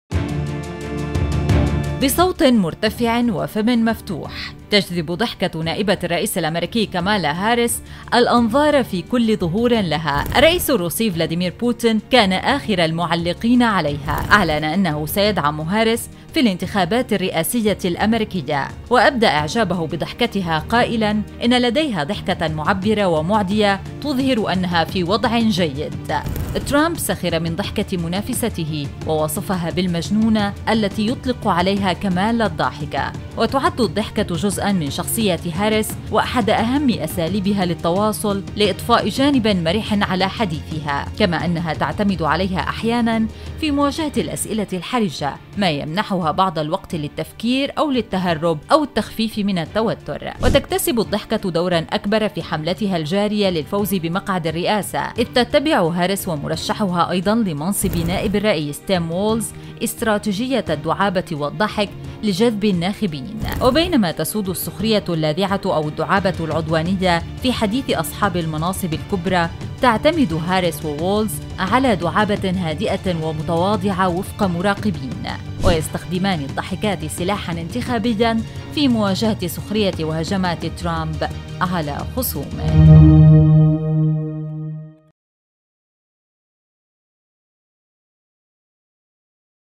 «ضحكة هاريس» تثير إعجاب «بوتين» و«جنون» ترامب!
يشيد بها بوتين ويراها "معبرة"، بينما ينتقدها ترامب ويصفها بـ "المجنونة".. ضحكة كامالا هاريس سلاحها الانتخابي في مواجهة هجمات خصمها